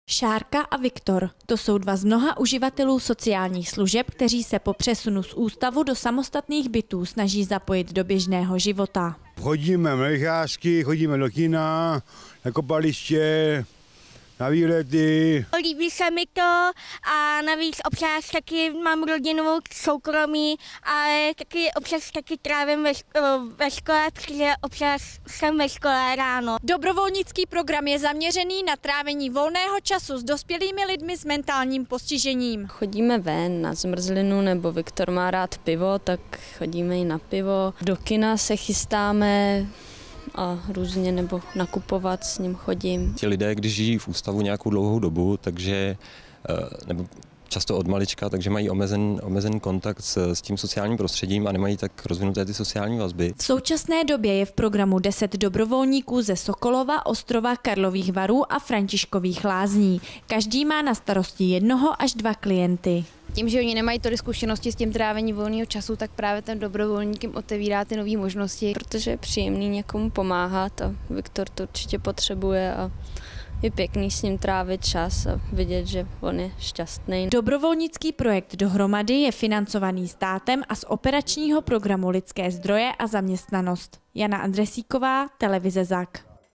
Dobrovolnický program DOHROMADY
Představení o.s. Instand. Transformace sociálních služeb pro lidi s mentálním postižením v Karlovarském kraji. Dobrovolnický program DOHROMADY zaměřený na trávení volného času s lidmi s mentálním postižením, kteří
Přecházejí z ústavní péče do samostatného bydlení. Zapojení cílové skupiny do běžného života.